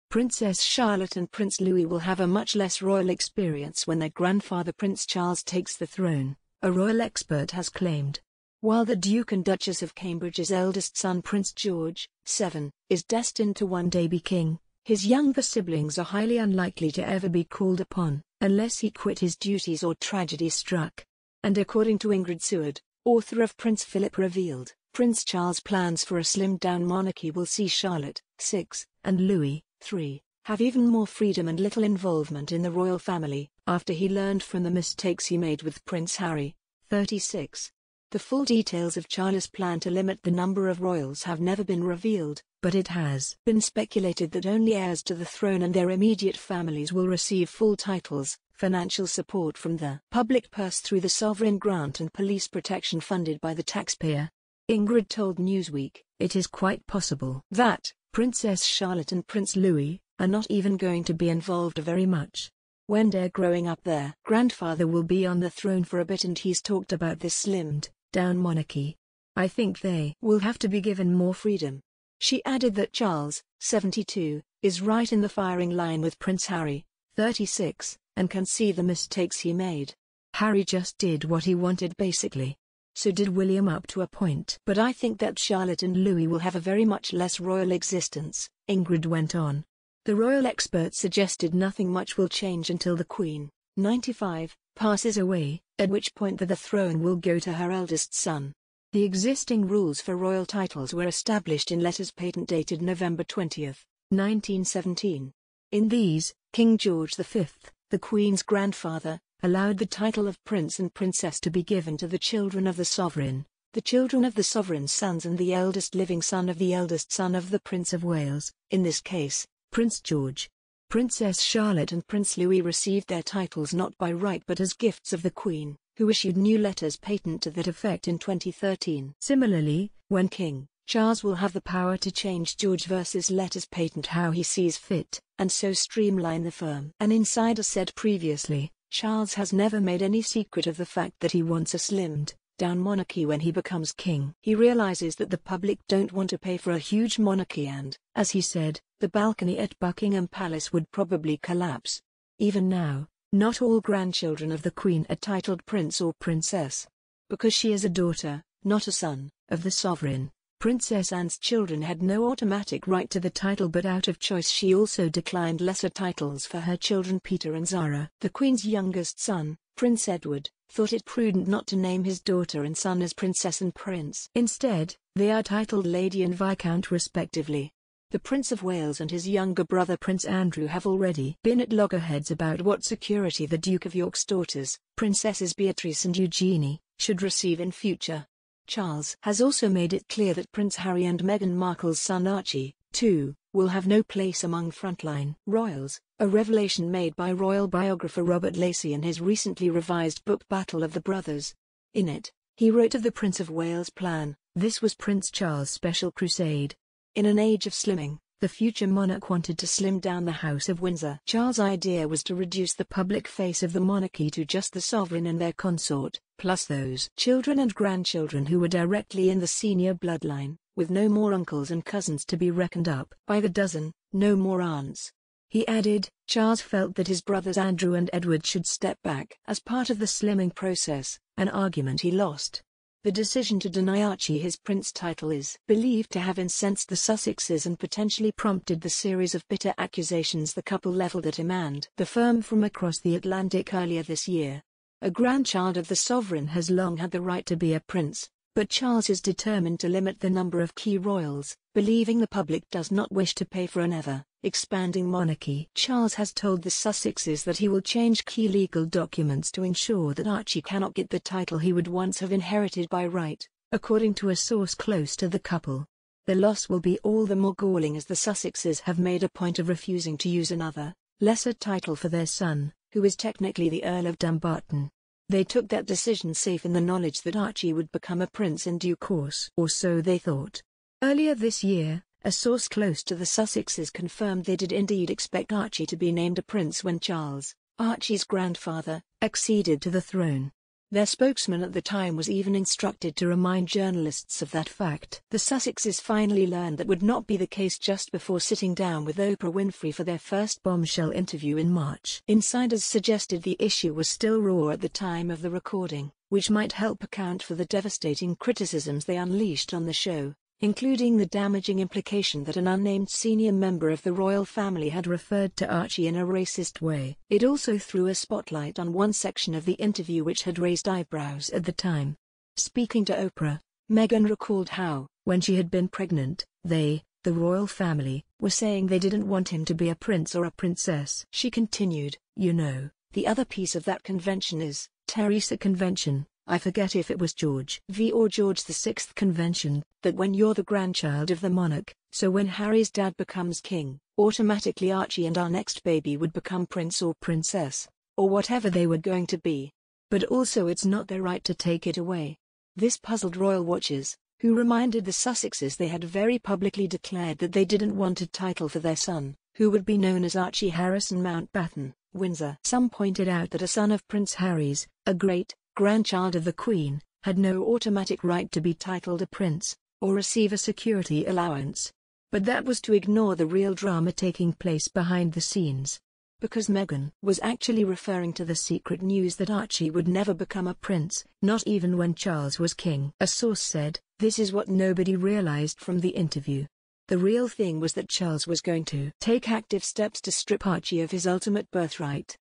Prince Charles has 'learned from the mistakes he made with Prince Harry' and wants Princess Charlo
Princess Charlotte and Prince Louis will have a 'much less royal experience' when their grandfather Prince Charles takes the throne, a royal expert has claimed. While the Duke and Duchess of Cambridge